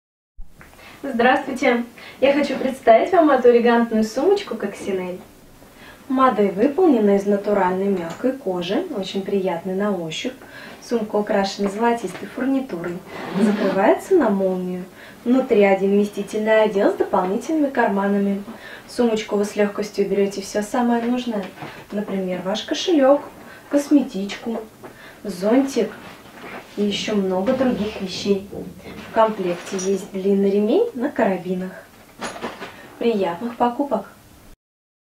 Сумка Coccinelle C1 UI0 18 01 01 221 turquoise
Превосходная сумка Coccinelle из высококачественной натуральной кожи - эффектная и универсальная модель для тех, кто все время в движении. Вместительный аксессуар закрывается на молнию, украшен золотистой фурнитурой. Размеры сумки позволят вместить в себя ежедневно необходимые для женщины предметы: косметику, документы и деловые бумаги формата А4, портмоне и всевозможные гаджеты, без которых не проходит ни один день современного городского жителя. Носится на сгибе руки или на плече.
Внутри: на задней стенке карман для документов на молнии, на передней стенке два кармана для мелочей.